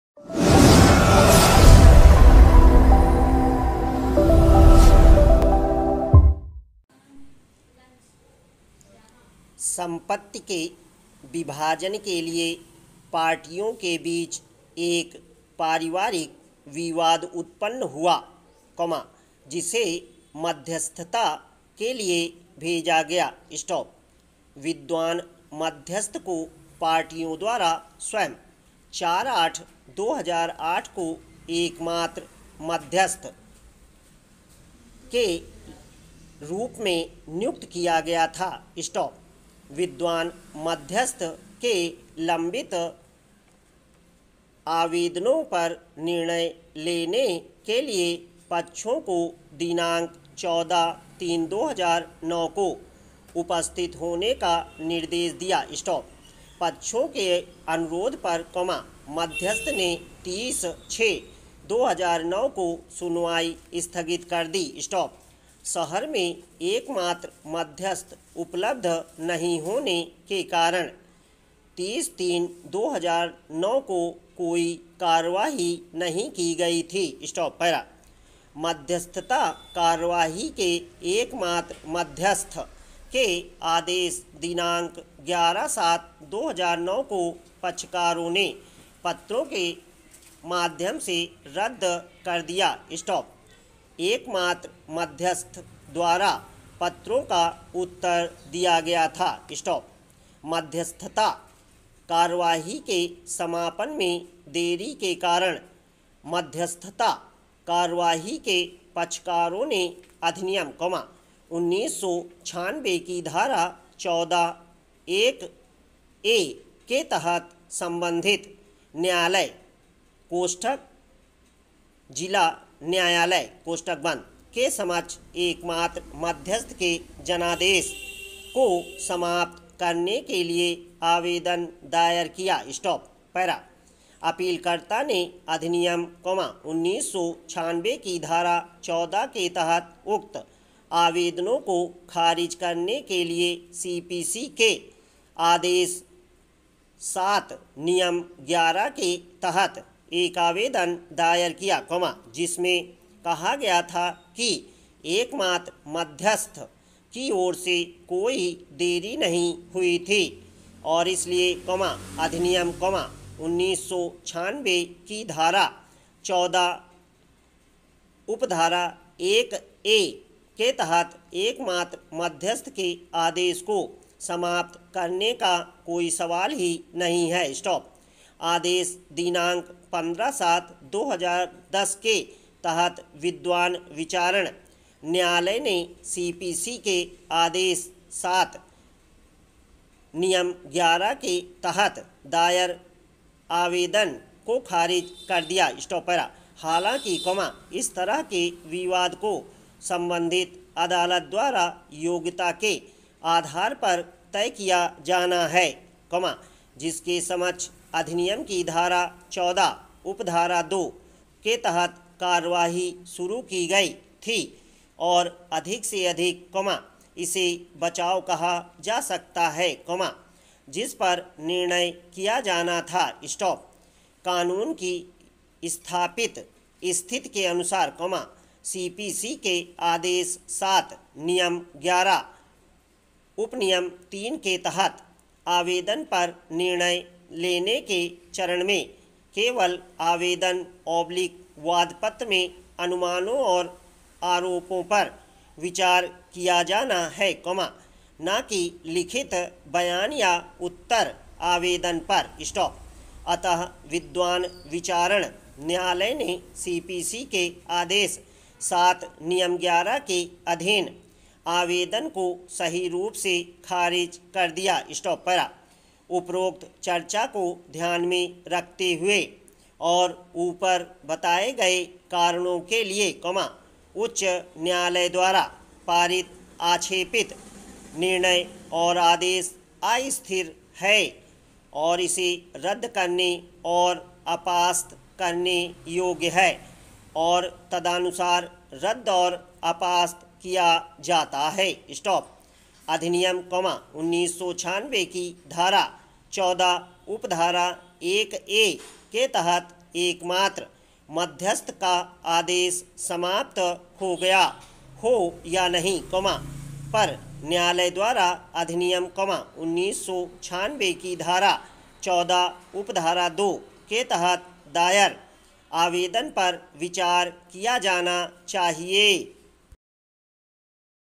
Hindi Legal Dictation for Allahabad High Court 80wpm
P-62
संपत्ति के विभाजन के लिए पार्टियों के बीच एक पारिवारिक विवाद उत्पन्न हुआ, जिसे मध्यस्थता के लिए भेजा गया। विद्वान मध्यस्थ को पार्टियों द्वारा स्वयं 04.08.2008 को एकमात्र मध्यस्थ के रूप में नियुक्त किया गया था। विद्वान मध्यस्थ ने लंबित आवेदनों पर निर्णय लेने के लिए पक्षों को दिनांक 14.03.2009 को उपस्थित होने का निर्देश दिया। पक्षों के अनुरोध पर, मध्यस्थ ने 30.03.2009 को सुनवाई स्थगित कर दी। शहर में एकमात्र मध्यस्थ उपलब्ध नहीं होने के कारण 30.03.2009 को कोई कार्यवाही नहीं की गई थी।(1)
 मध्यस्थता कार्यवाही के एकमात्र मध्यस्थ के आदेश दिनांक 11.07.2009 को पक्षकारों ने पत्रों के माध्यम से रद्द कर दिया। एकमात्र मध्यस्थ द्वारा पत्रों का उत्तर दिया गया था। मध्यस्थता कार्यवाही के समापन में देरी के कारण मध्यस्थता कार्यवाही के पक्षकारों ने अधिनियम, 1996 की धारा 14(1)(ए) के तहत संबंधित न्यायालय (जिला न्यायालय) के समक्ष एकमात्र मध्यस्थ के जनादेश को समाप्त करने के लिए आवेदन दायर किए।
 अपीलकर्ता ने अधिनियम, 1996 की धारा 14 के तहत उक्त आवेदनों को खारिज करने के लिए सीपीसी के आदेश (2) VII नियम 11 के तहत एक आवेदन दायर किया, जिसमें कहा गया था कि एकमात्र मध्यस्थ की ओर से कोई देरी नहीं हुई थी और इसलिए, अधिनियम, 1996 की धारा 14(1)(ए) के तहत एकमात्र मध्यस्थ के आदेश को समाप्त करने का कोई सवाल ही नहीं है। आदेश दिनांक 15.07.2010 के तहत विद्वान विचारण न्यायालय ने सीपीसी के आदेश VII नियम 11 के तहत दायर आवेदन को खारिज कर दिया। हालांकि, इस तरह के विवाद को संबंधित अदालत द्वारा योग्यता के आधार पर तय किया जाना (3) है, जिसके समक्ष अधिनियम की धारा 14(2) के तहत कार्यवाही शुरू की गई थी और अधिक से अधिक, इसे बचाव कहा जा सकता है, जिस पर निर्णय किया जाना था। कानून की स्थापित स्थिति के अनुसार, सीपीसी के आदेश VII नियम 11(3) के तहत आवेदन पर निर्णय लेने के चरण में केवल आवेदन/वादपत्र में अनुमानों और आरोपों पर विचार किया जाना है, न कि लिखित बयान या उत्तर आवेदन पर। अत: विद्वान विचारण न्यायालय ने सीपीसी के आदेश VII नियम 11 के अधीन आवेदन को (4) सही रूप से खारिज कर दिया।
 उपरोक्त चर्चा को ध्यान में रखते हुए और ऊपर बताए गए कारणों के लिए, उच्च न्यायालय द्वारा पारित आक्षेपित निर्णय और आदेश अस्थिर है और इसे रद्द करने और अपास्त करने योग्य है और तदनुसार रद्द और अपास्त किया जाता है। चअधिनियम, 1996  की धारा 14(1)(ए) के तहत एकमात्र मध्यस्थ का आदेश समाप्त हो गया हो या नहीं, पर न्यायालय द्वारा अधिनियम, 1996 की धारा 14(2) के तहत दायर आवेदन पर विचार किया जाना चाहिए।